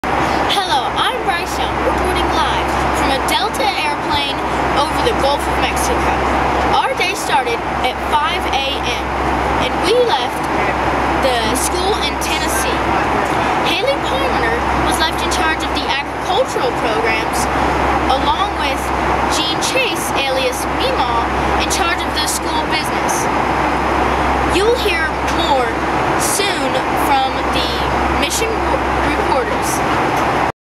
Here we are heading to Honduras
School Mission Trip to Honduras March 1, 2008